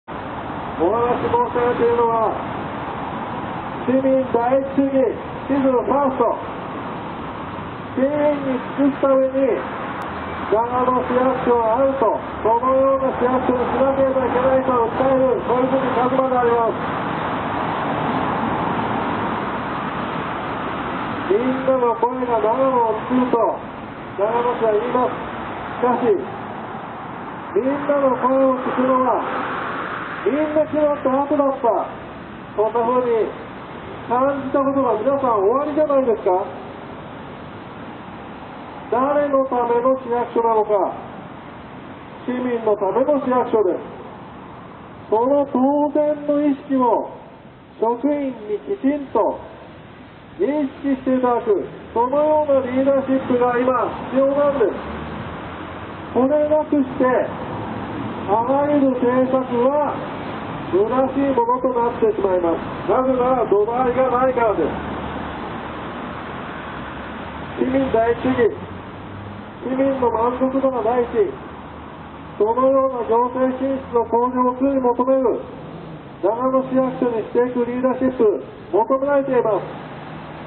シチズン・ファースト―市民第一主義は全ての政策の土台
市民のために市役所はある。「みんなの声がながのをつくる」と言いながら、みんなの声を聴くのは、みんな決まった後。お役所の都合第一主義から、市民第一主義へ。それなくして、あらゆる政策・制度は虚しい。シチズン・ファーストは、市民のための市役所を実効あらしめるための、土台なのです。実現させる強いリーダーシップが、求められています。